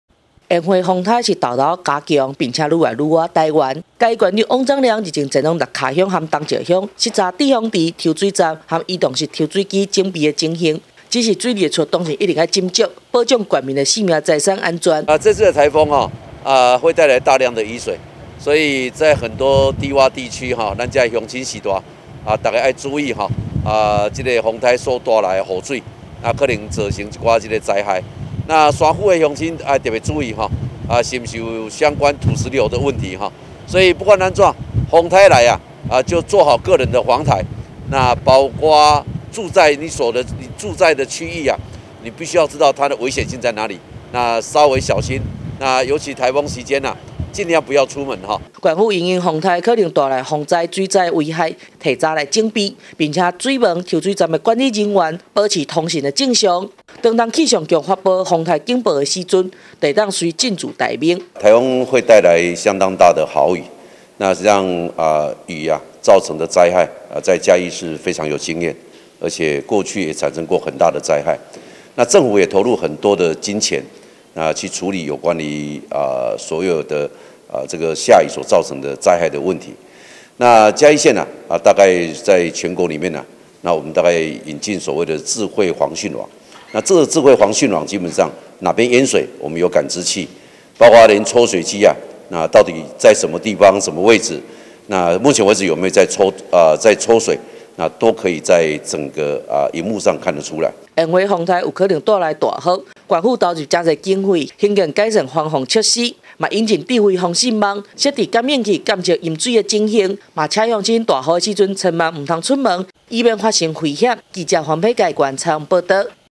烟花颱風北移不輕忽 翁章梁視察防汛整備-大揚新聞
烟花颱風逐步增強並逼近台灣，
嘉義縣長翁章梁日前前往六腳鄉和東石鄉視察滯洪池、
抽水站和移動式抽水機整備情形，
指示水利處同仁務必嚴陣以待，
保障縣民生命財產安全。